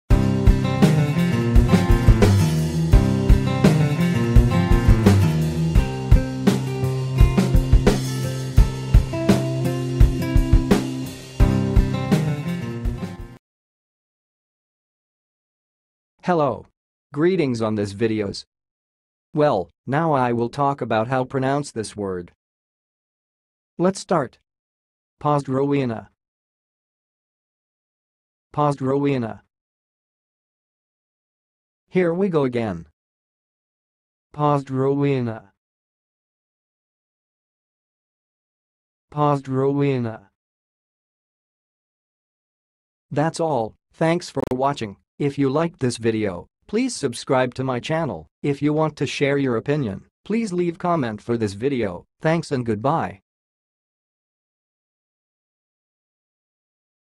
HOW TO SAY POZDROWIENA?
POZDROWIENA - Pozdrowiena: What it is and how to pronounce it correctly in 2023

POZDROWIENA Meaning And Definition:
Pozdrowienia (polish for greetings) is a general term used to greet someone or express good wishes.
It can be used in various contexts and situations.
POZDROWIENA Pronunciation:
To pronounce 'pozdrowienia' in Polish, you can break it down into syllables: 'poz-dro-wie-nia'.
Each syllable is pronounced with equal stress and the 'a' at the end is nasalized.
Alternatively, you can listen to audio samples online for correct pronunciation.
Use POZDROWIENA In Your Speech:
1. Dzień dobry!
2. Jak się masz?
3. Pozdrowienia od całej rodziny
4. Cześć!
5. Cieszę się, że cię widzę.
6. Pozdrowienia z Warszawy!
7. Witam serdecznie!
8. Wszystkiego dobrego z okazji urodzin
9. Hej, stary!
10. Długo się nie widzieliśmy.
11. Pozdrowienia ode mnie i moich kolegów
12. Dobry wieczór!
13. Życzę miłego wieczoru i udanej nocy.
14. Pozdrowienia dla twoich bliskich
Please answer in comments for the most popular POZDROWIENA questions:
1. What are some common greetings in Poland?
2. How do Polish people greet each other formally?
3. Are there any specific greeting rituals in different regions of Poland?
4. What are some phrases to express good wishes in Polish?
5. How should I greet someone in a business setting in Poland?
6. What are the cultural greeting customs in Polish society?
7. How do Poles greet each other during holidays and celebrations?